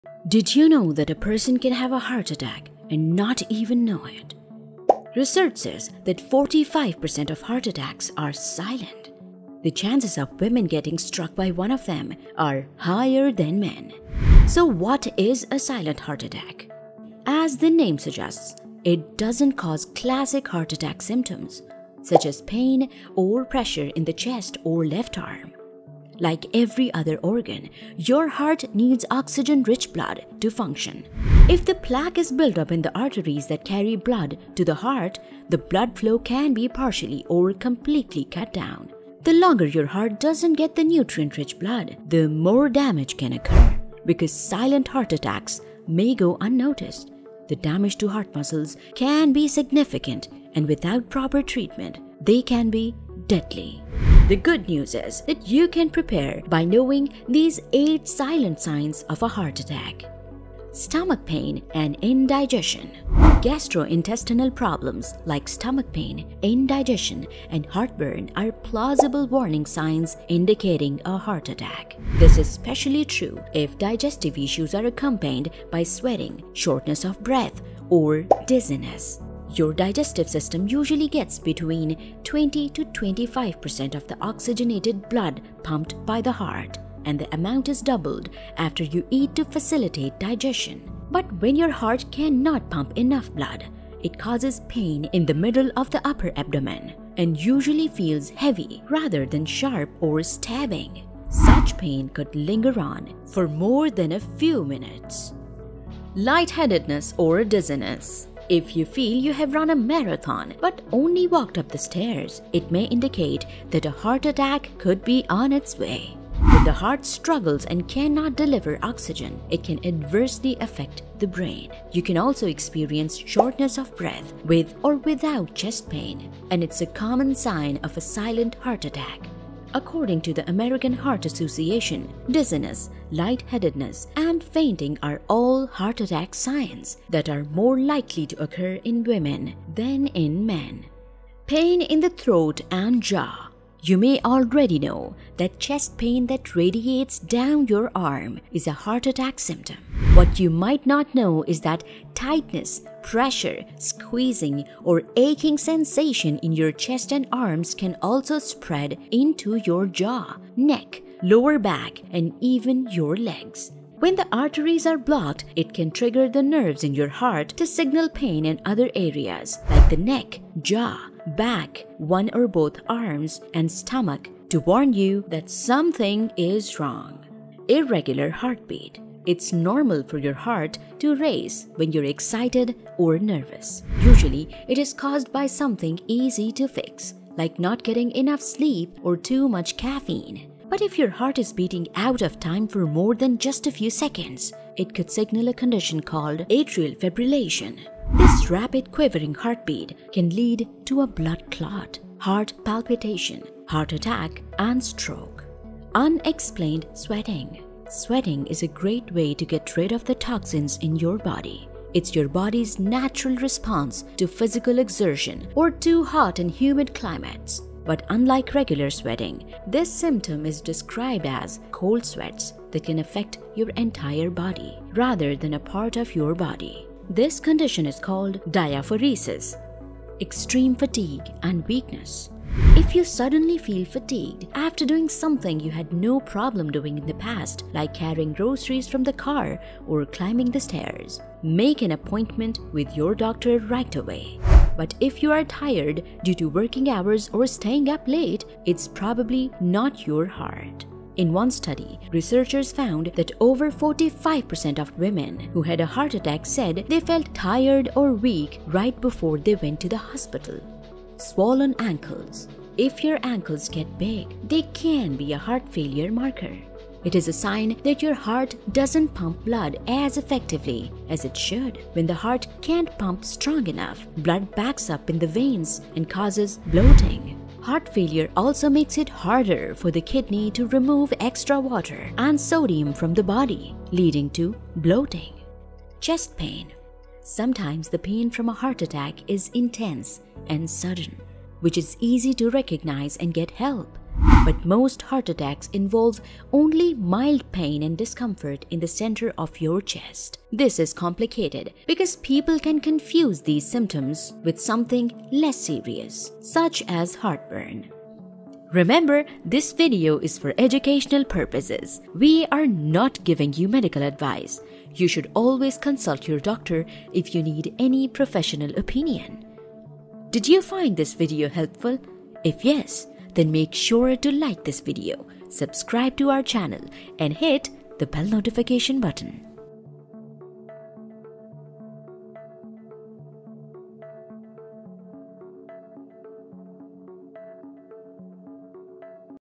8 Signs Of A Silent Heart Attack That Are Always Ignored
Did you know a person can have a heart attack and not even know it? Research says that 45% of heart attacks are silent. The chances of women getting struck by one of them are higher than men. So, what is a silent heart attack?

As the name suggests, it doesn't cause classic heart attack symptoms, such as pain or pressure in the chest or left arm. Because silent attack attacks may go unnoticed, the damage to heart muscles can be significant, and without proper treatment, they can be deadly.

⌛Timestamps:
⏱️0:00 What Is Silent Heart Attack
⏱️1:00 Stomach Pain & Indigestion
⏱️1:46 Lightheadedness or Dizziness
⏱️2:21 Pain in the throat and jaw
⏱️2:57 Irregular heartbeat
⏱️3:25 Unexplained sweating
⏱️3:50 Extreme Fatigue and weakness
⏱️4:21 Swollen ankles
⏱️4:45 Chest pain

✍️ Summary:

Gastrointestinal problems like stomach pain, indigestion, and heartburn are plausible warning signs indicating a heart attack. This is especially true if digestive issues are accompanied by sweating, shortness of breath, or dizziness.

If you feel you have run a marathon but only walked up the stairs, it may indicate that a heart attack could be on its way. When the heart struggles and cannot deliver oxygen, it can adversely affect the brain.

You may already know that chest pain that radiates down your arm is a heart attack symptom. What you might not know is that tightness, pressure, squeezing, or aching sensation in your chest and arms can also spread into your jaw, neck, lower back, and even your legs.

If your heart is beating out of time for more than just a few seconds, it could signal a condition called atrial fibrillation.

Unlike regular sweating, this symptom is described as cold sweats that can affect your entire body rather than a part of your body. This condition is called diaphoresis.

If you suddenly feel fatigued after doing something you had no problem doing in the past, like carrying groceries from the car or climbing the stairs, make an appointment with your doctor right away.

If your ankles get big, they can be a heart failure marker. It is a sign that your heart doesn't pump blood as effectively as it should.

Sometimes, the pain from a heart attack is intense and sudden, which is easy to recognize and get help. But most heart attacks involve only mild pain or discomfort in the center of your chest.